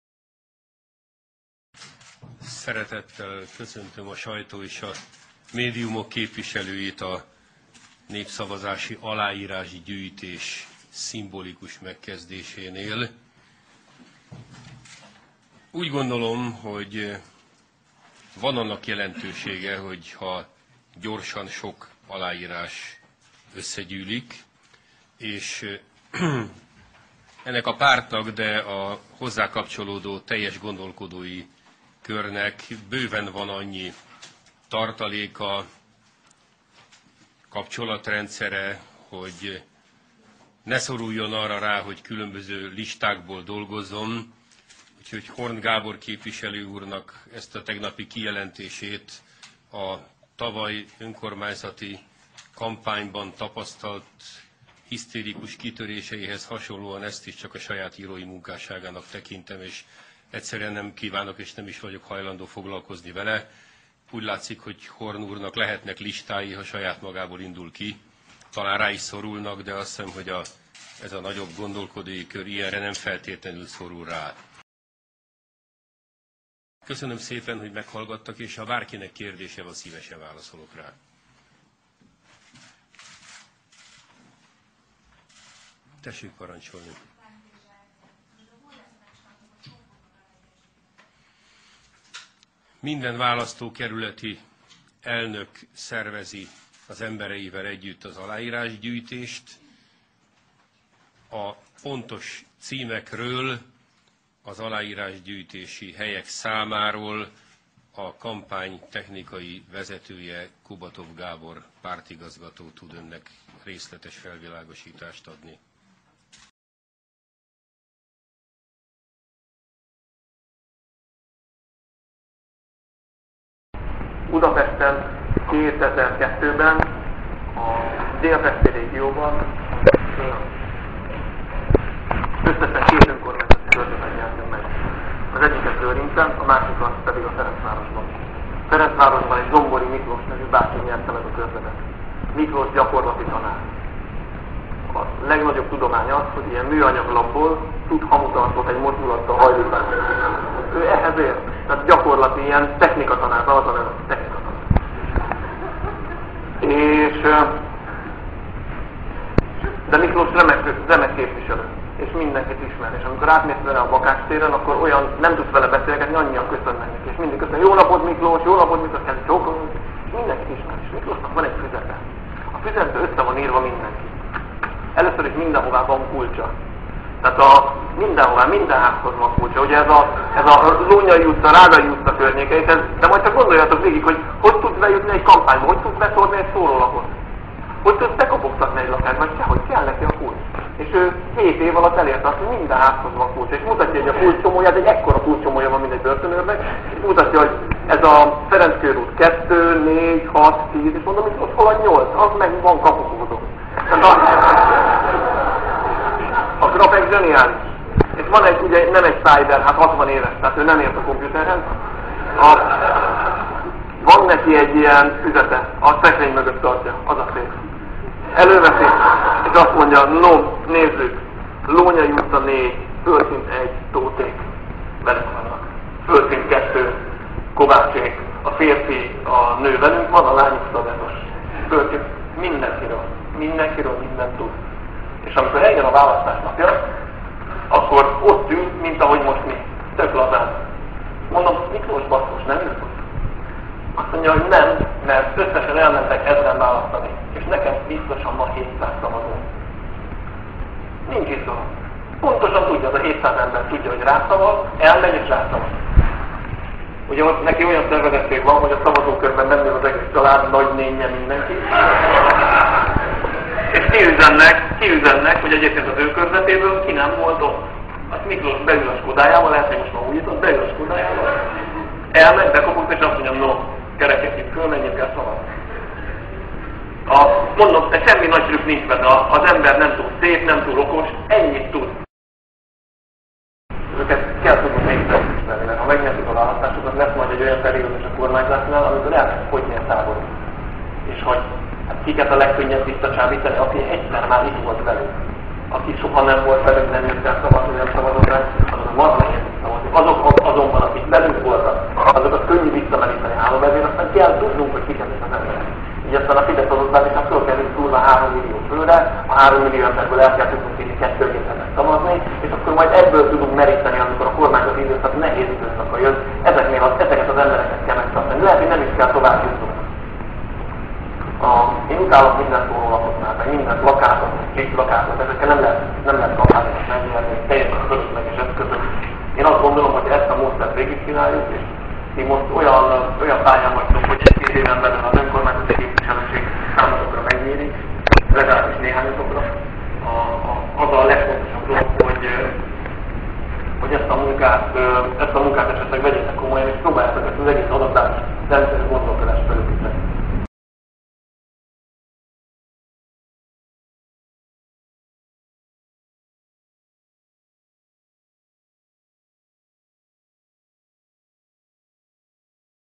Tarlós István nincs adatbázis, kérdezzék Kubatov Gábort
Tarlós és az újabb Kubatov felvétel, Tarlós szerint Kubatov több mindent tud...